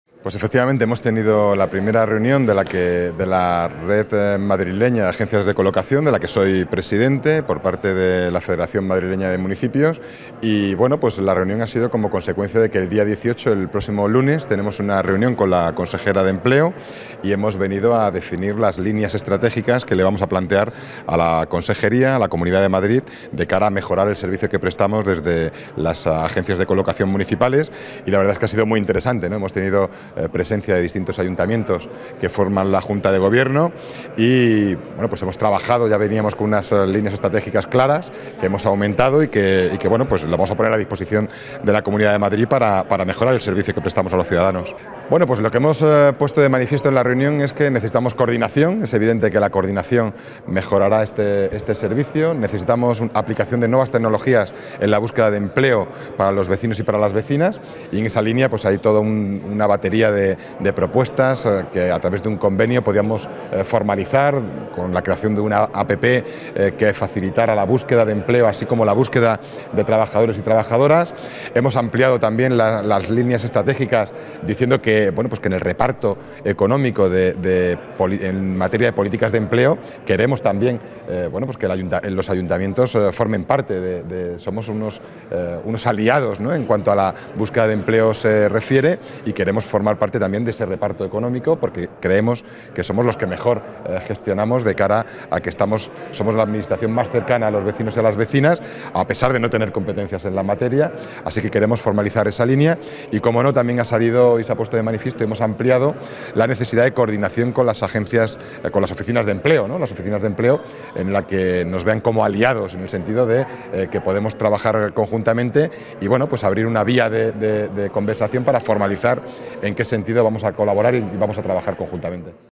Fuenlabrada acoge la primera reunión de la RECMA
La Red de Agencias Municiaples de Colocación y Empleo de la Comunidad de Madrid, perteneciente a la Federación de Municipios de Madrid, ha celerbado su primera reunión en Fuenlabrada.
El alcalde, Javier Ayala, preside esta Red y hoy ha sido el anfitrión de este encuentro en el que han preparado las líneas estratégicas para la reunión que celebrarán la próxima semana con la consejera de Empleo del Gobierno regional, Engracia Hidalgo